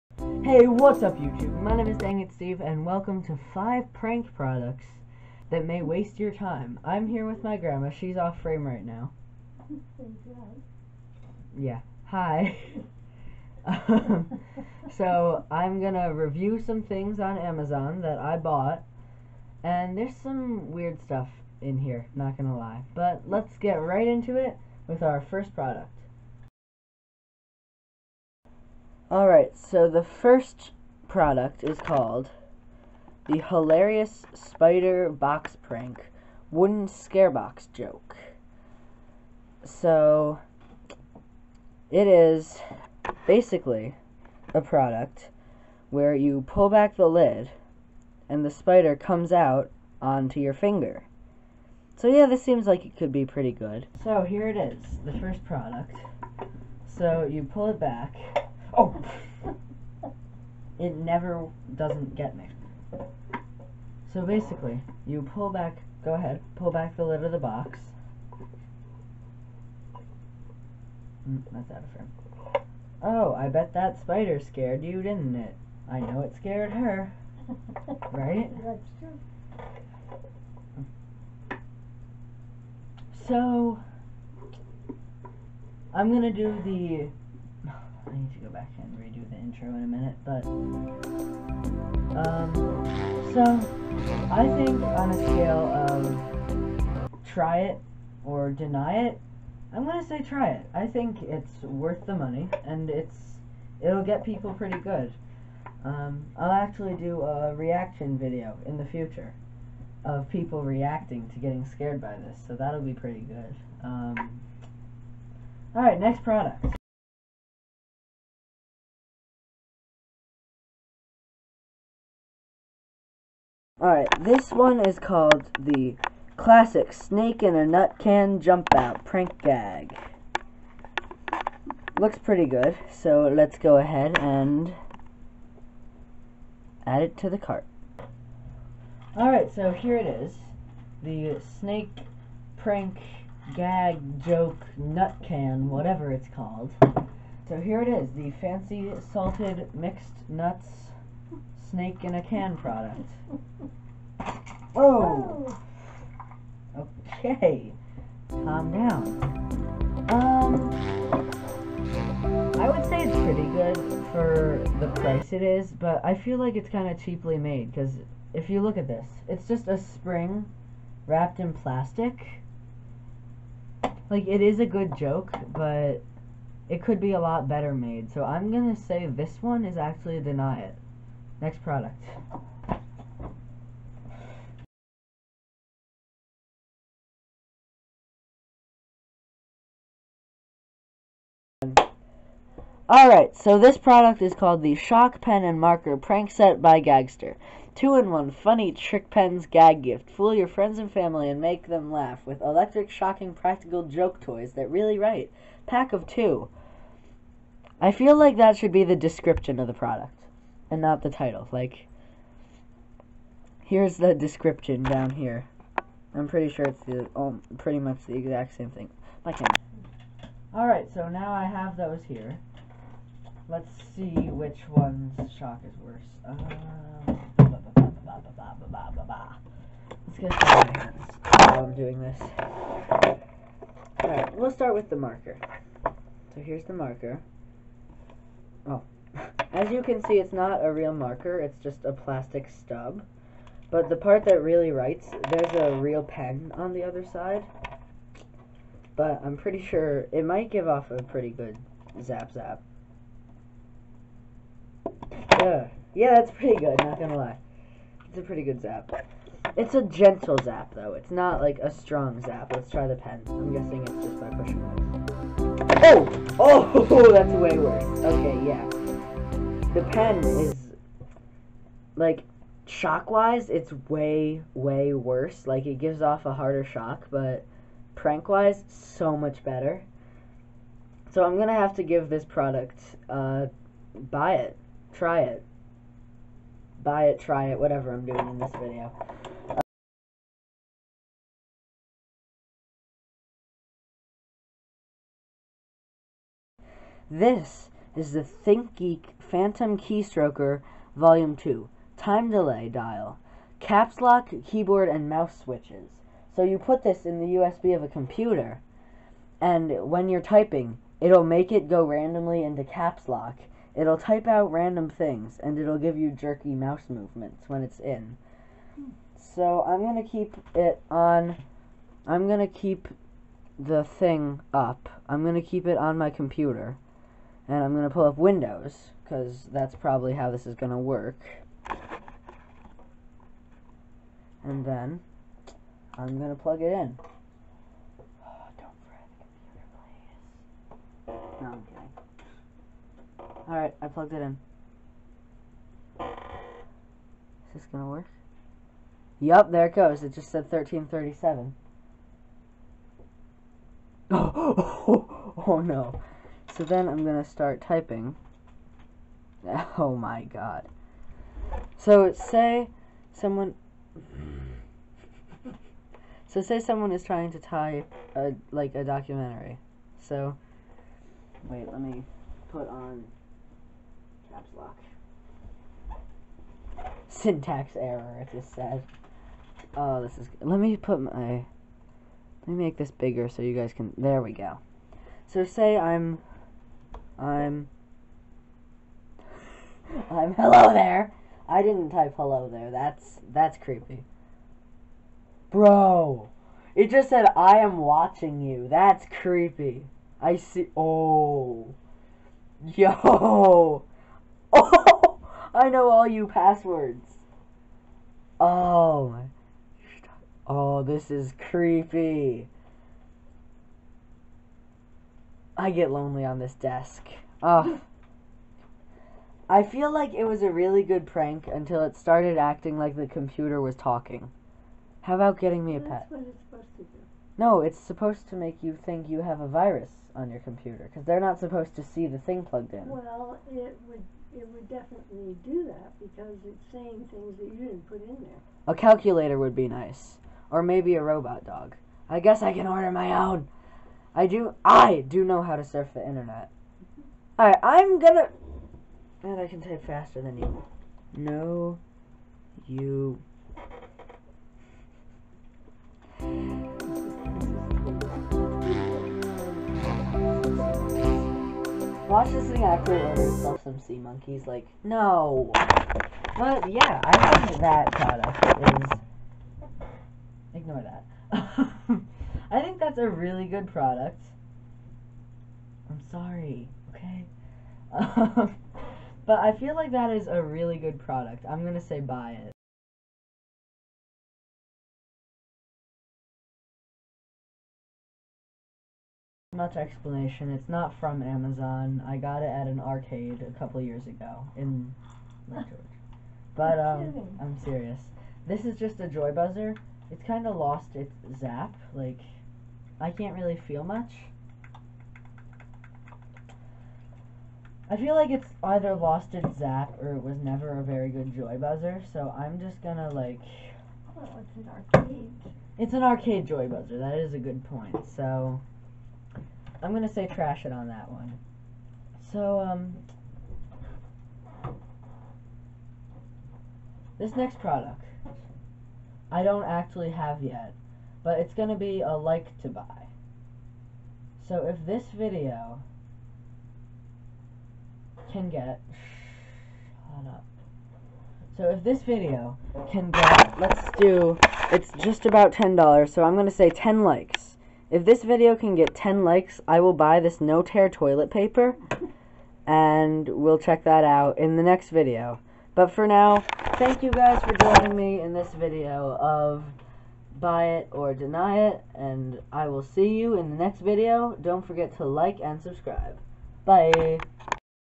SPIDER IN A BOX?!?!?! | 5 Prank products thay may wate your time
Today I bought some strange things that  are used for pranks off of Amazon.  I AM NOT MATTHIAS OR COPYING MATTHIAS!!!